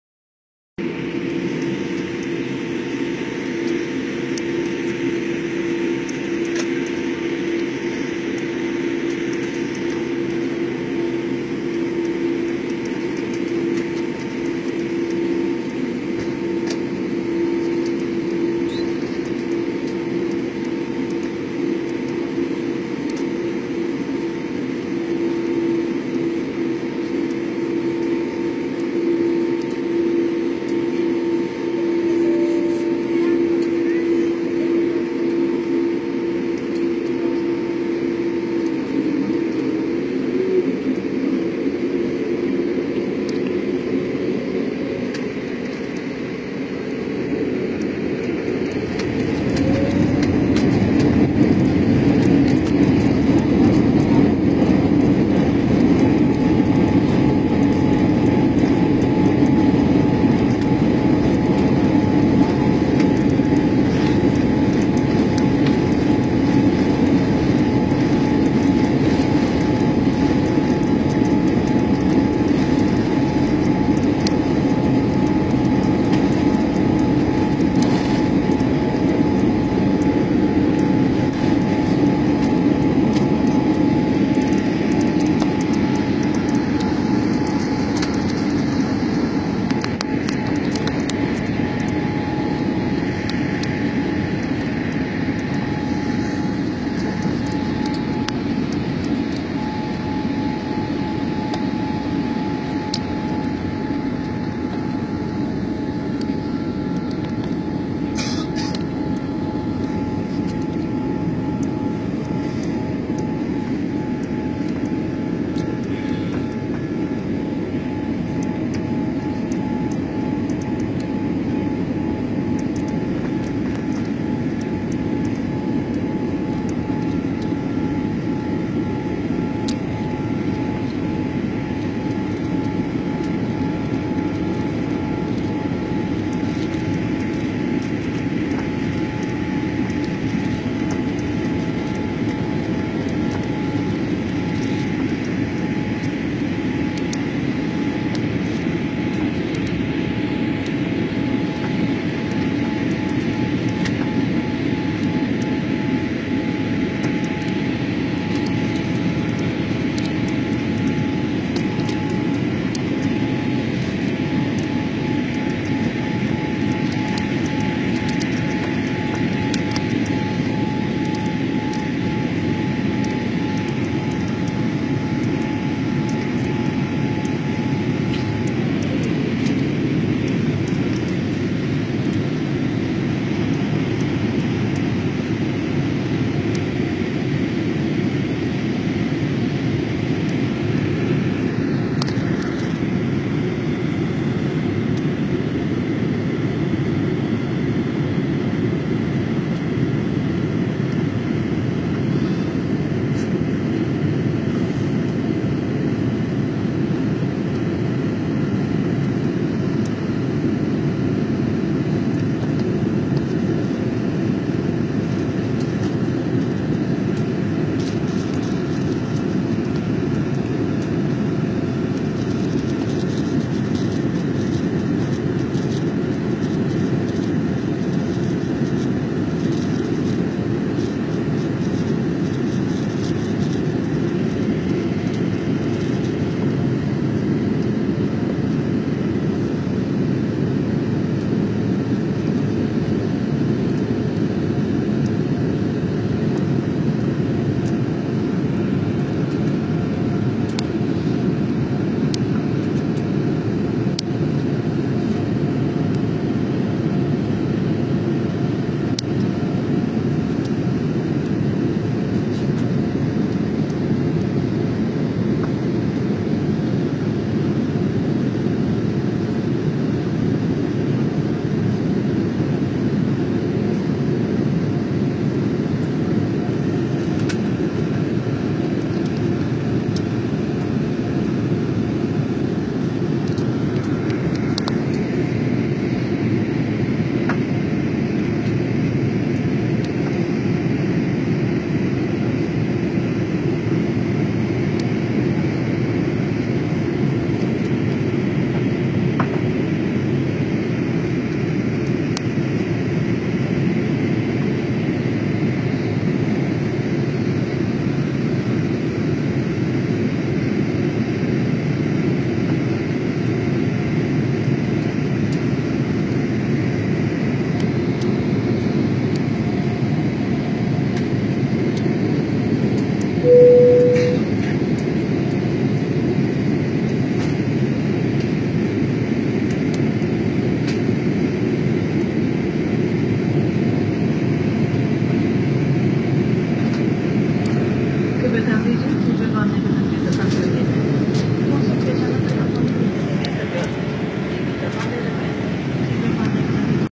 chenNai to singapore flight take off
Marina | Kathipara | Nehru stadium | full chennai from aerial view 😍😘😘